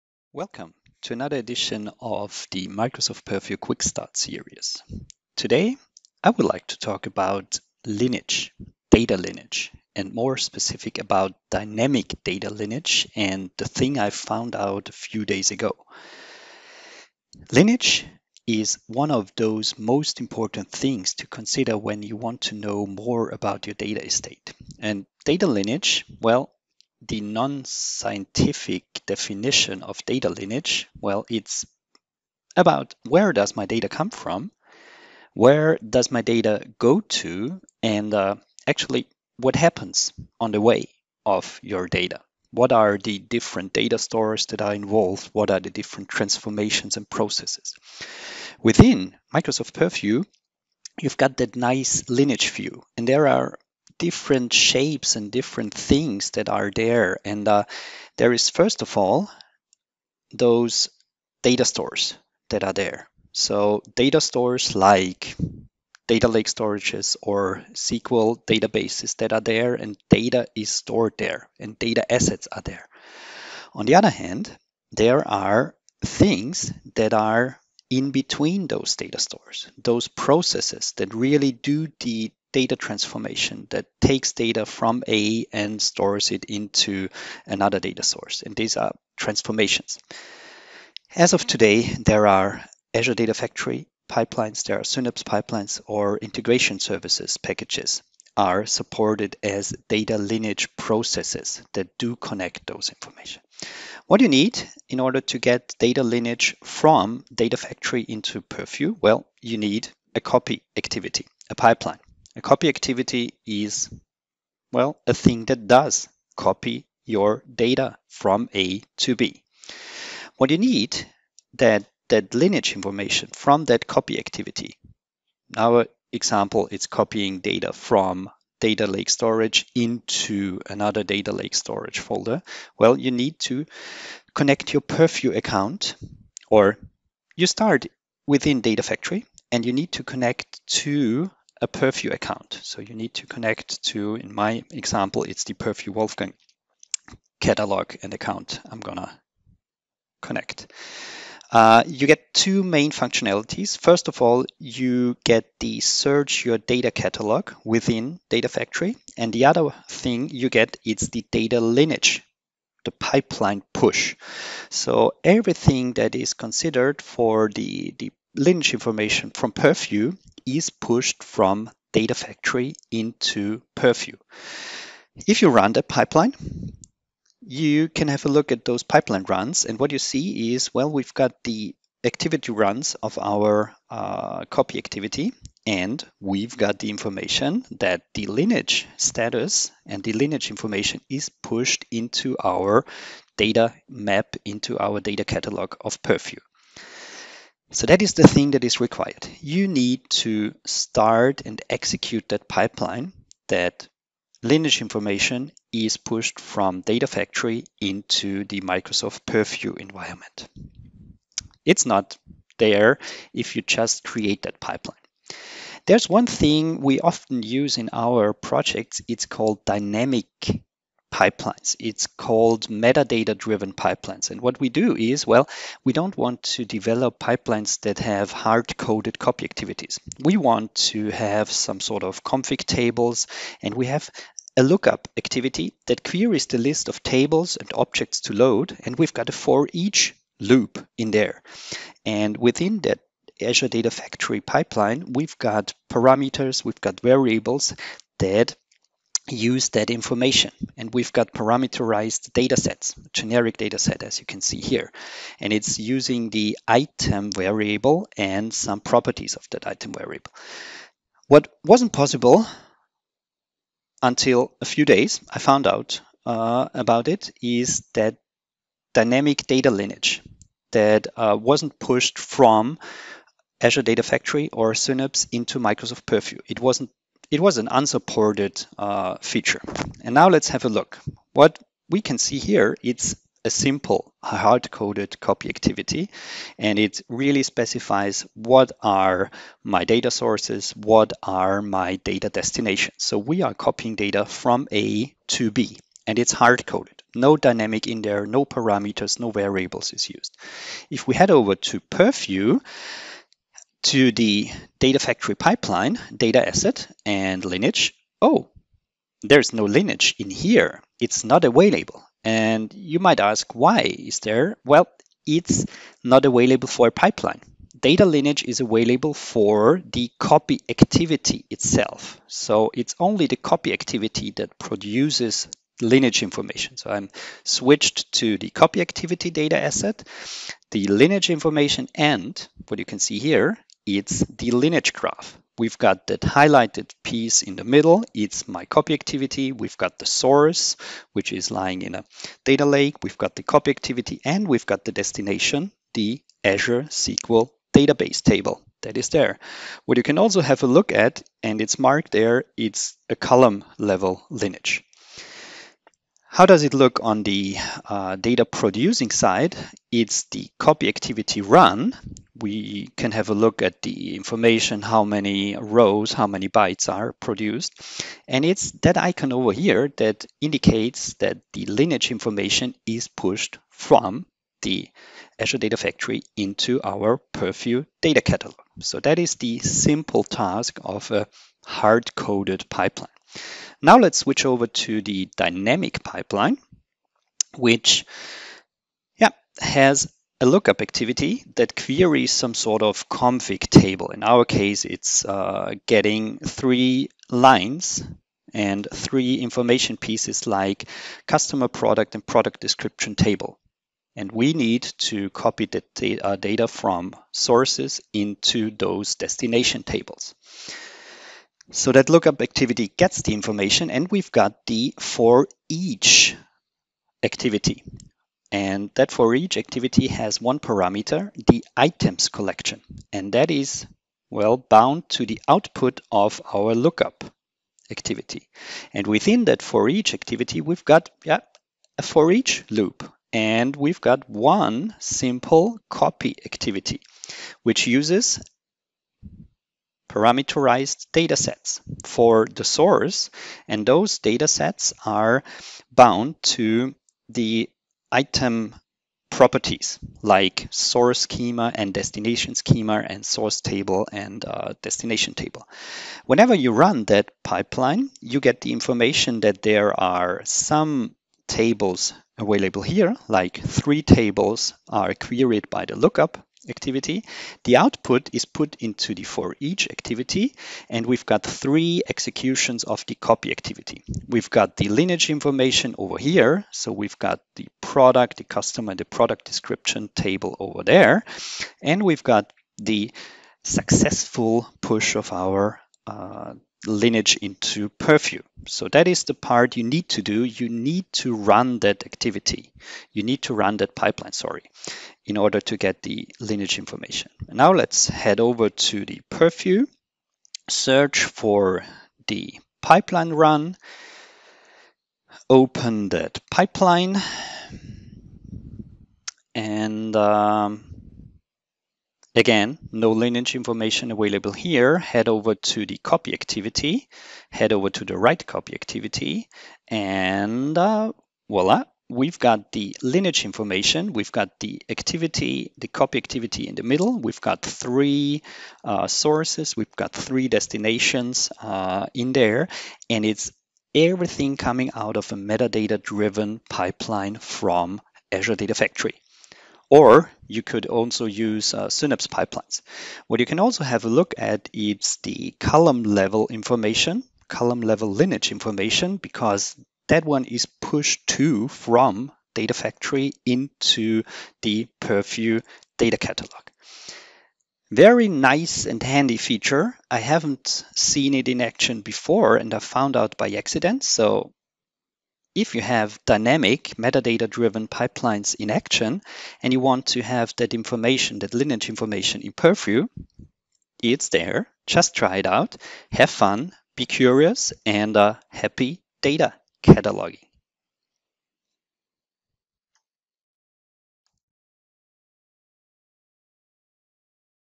Microsoft Purview Quickstart #13 - Lineage for dynamic ADF / Synapse pipelines
Data Lineage is already there in Azure Purview, right? Data lineage from Azure Data Factory (ADF) / Synapse copy activities got pushed into Purview? But there is something new and nice available right now - if you use metadata driven pipelines in ADF, you now get all the lineage information in Purview. Which is really nice! ;-)

In this video, I will guide you through the concept of lineage connection between ADF/Synapse and Purview and how dynamic pipeline runs are now represented in the Microsoft Purview lineage graph.